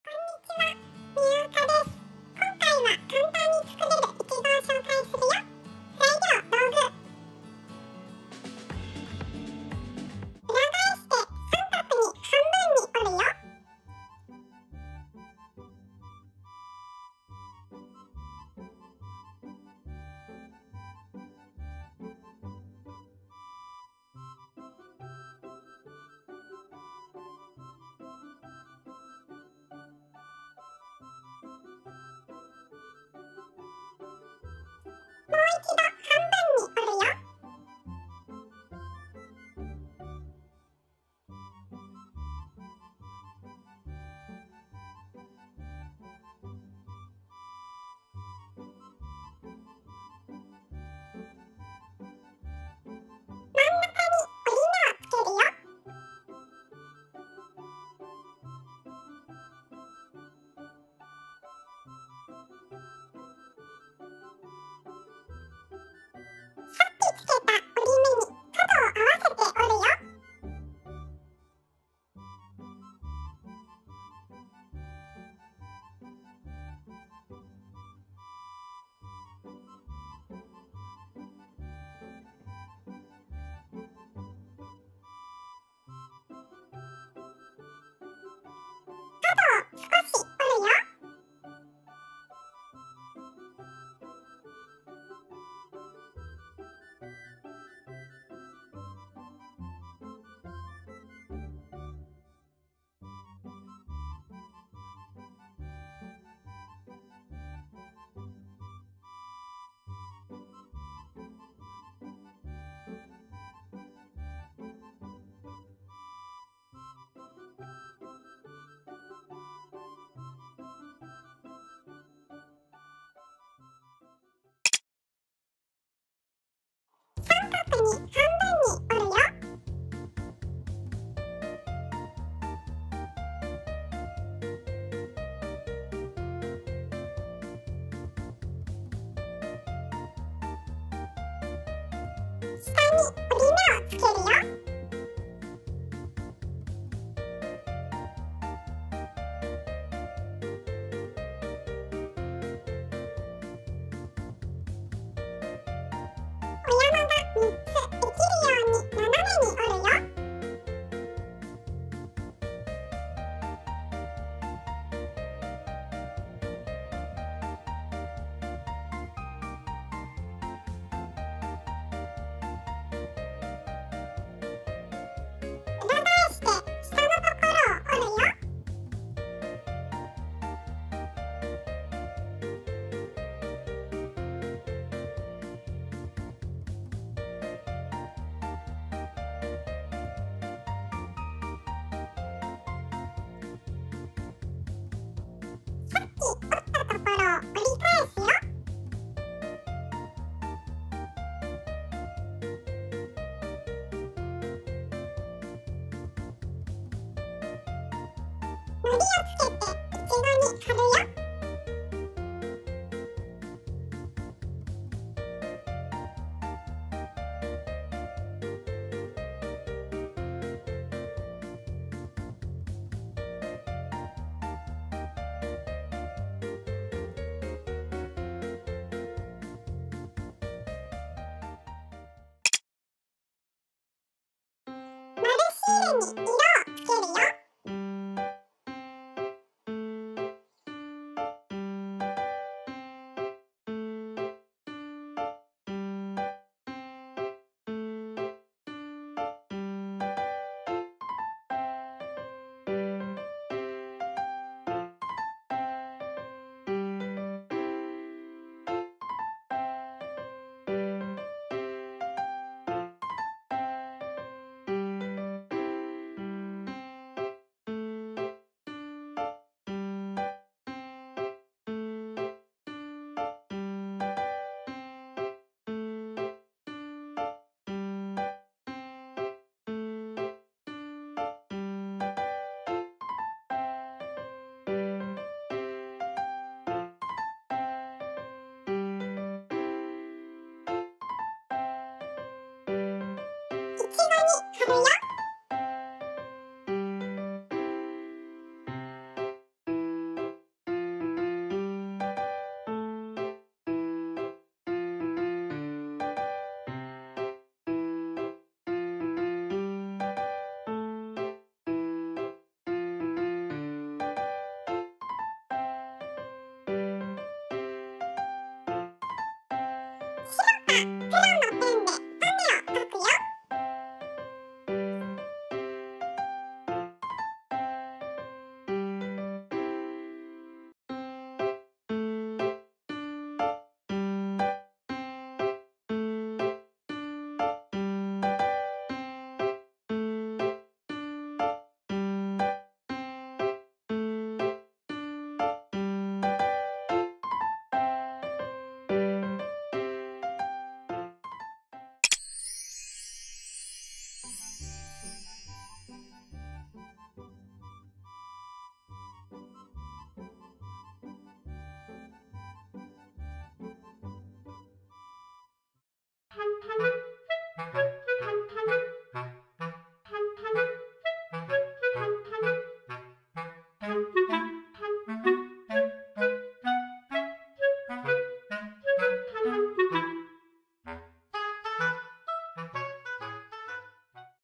【かんたん折り紙】小さい子もおれる！かわいい♡いちご｜Origami strawberry
こんにちは！みゅーあかです。

春の楽しみといえばいちご狩り♡
今回はとってもかんたんにできるいちごのおりかたをしょうかいするよ！

00:00完成イメージ
00:09材料
00:11いちご
02:06はっぱ
03:54かお
05:52かんせい